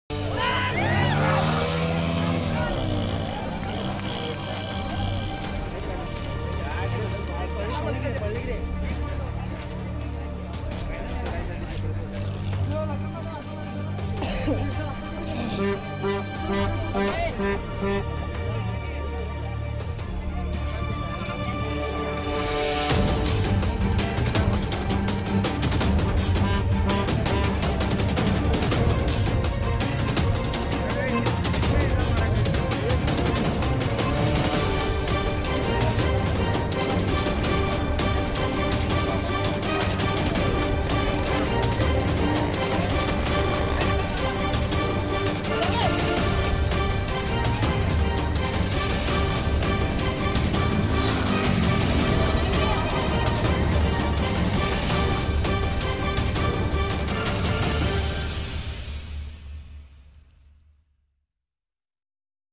संकेश्वर श्वान शर्यत Sankeshwar dog Race !!!, Kolhapur dog racing club 🐕 💐
संकेश्वर येथे 2019 ला यात्रेत झालेल्या मठ गल्ली आयोजित